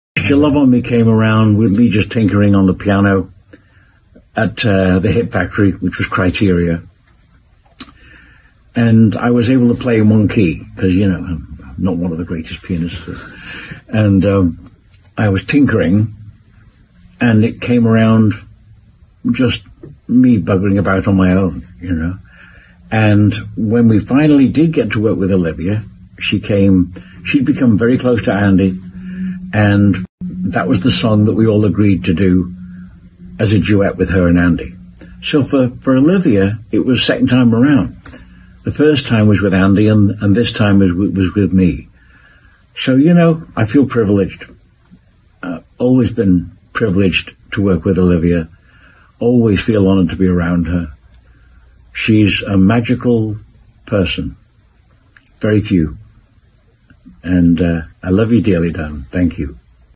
Barry Gibb talks Olivia Newton-John working on duets & Andy Gibb
Barry Gibb of the Bee Gees talks about working with Olivia Newton-John, first with Andy Gibb and then working with himself, in a 2021 interview.